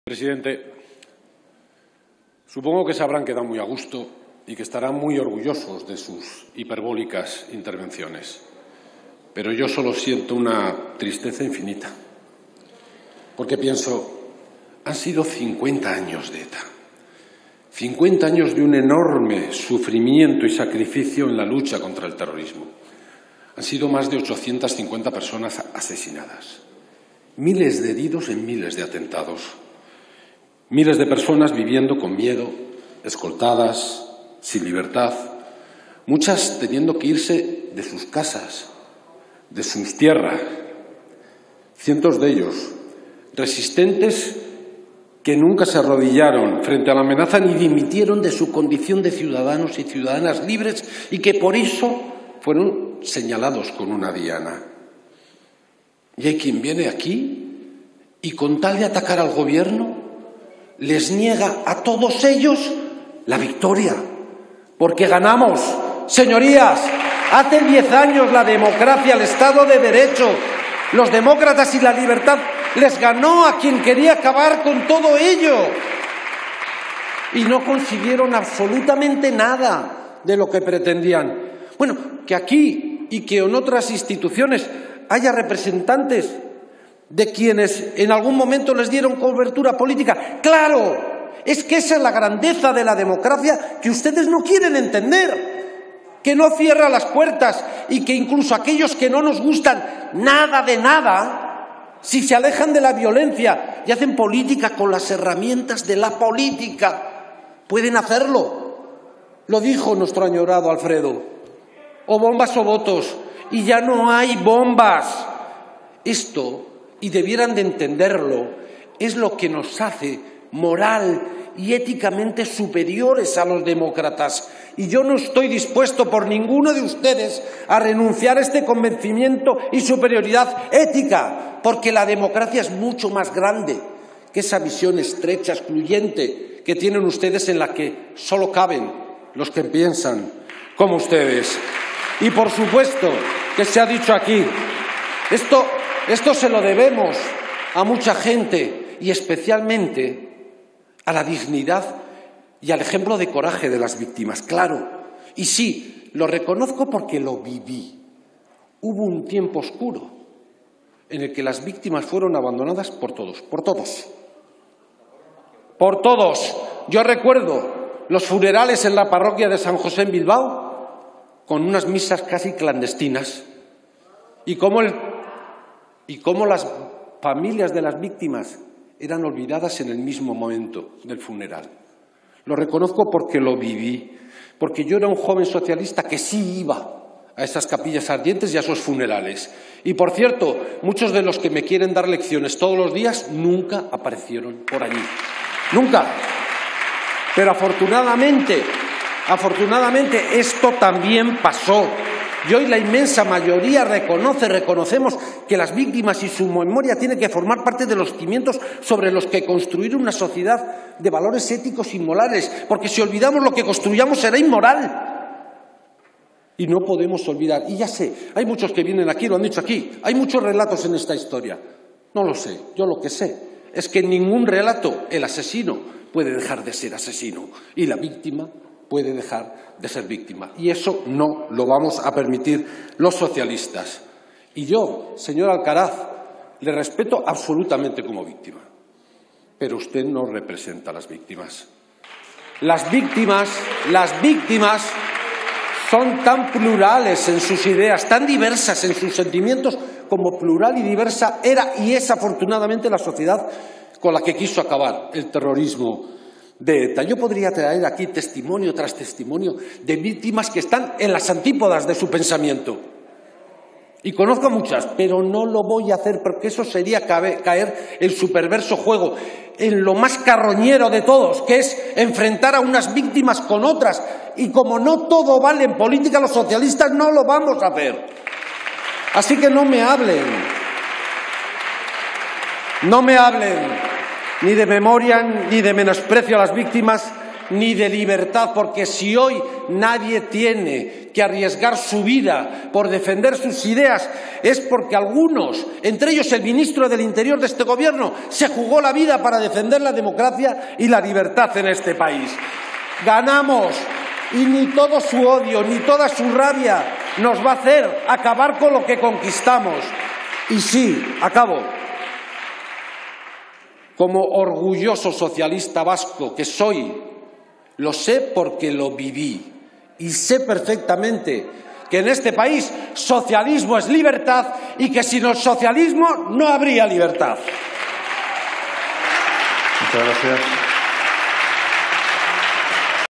Patxi López: "Socialismo es libertad. En este país, sin socialismo no habría libertad"
Con tal de atacar al Gobierno, hay quien es capaz de negar la victoria que la democracia logró hace ya diez años sobre los terroristas.
Por eso, hoy vivimos en libertad. ¡Porque ganamos!
En este país, socialismo es libertad.
En este país, sin socialismo no habría libertad.